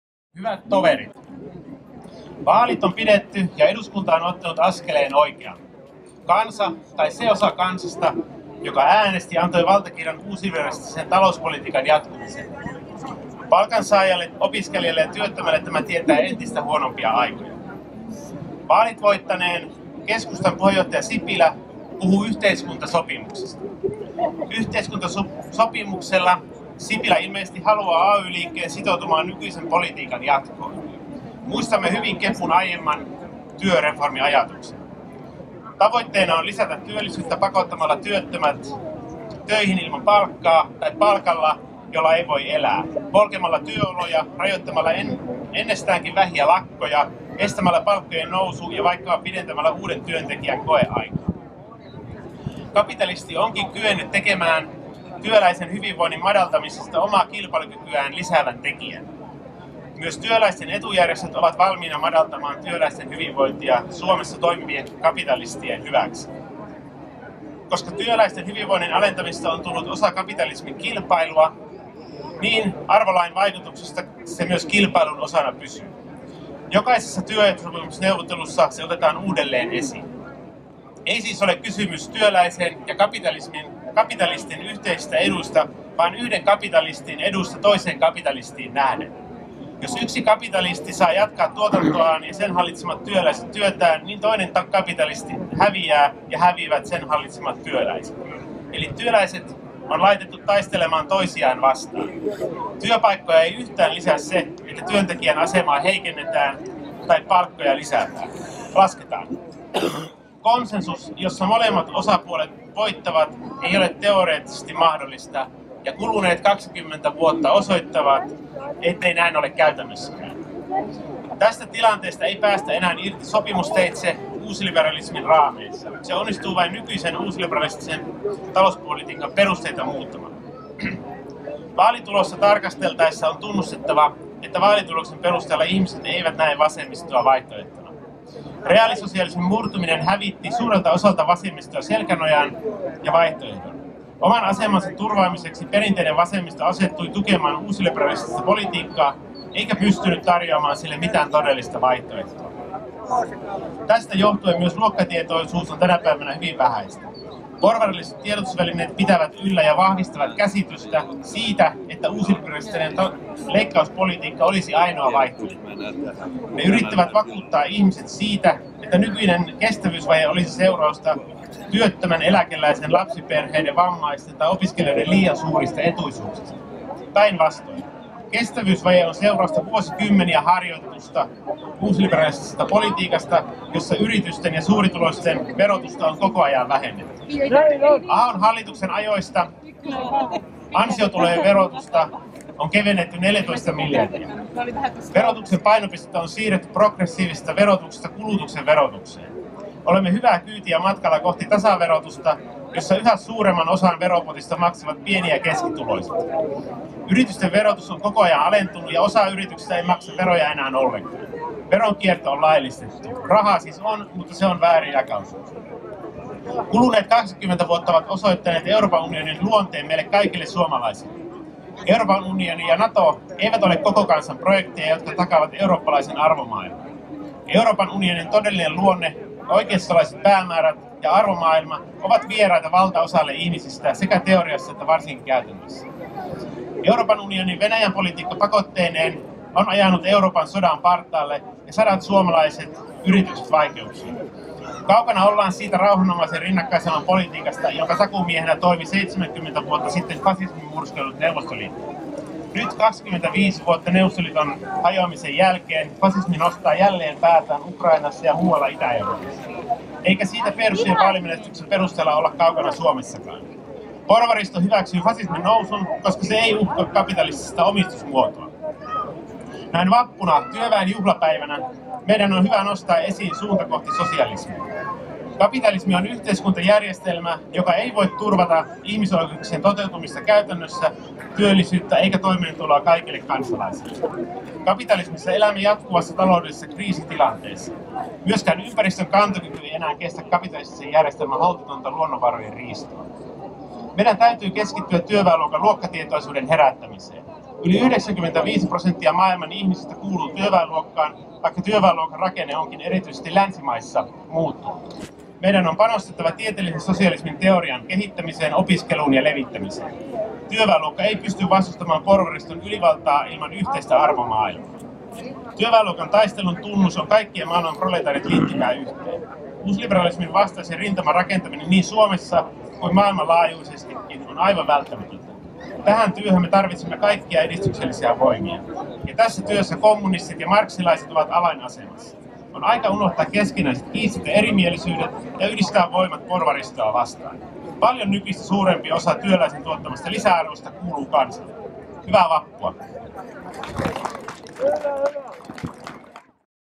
Kapitalismi tarjoaa vain heikennyksiä
Juha Kieksin (stp) vappupuhe Tokoinrannan työläisten ja opiskelijoiden juhlassa 2015.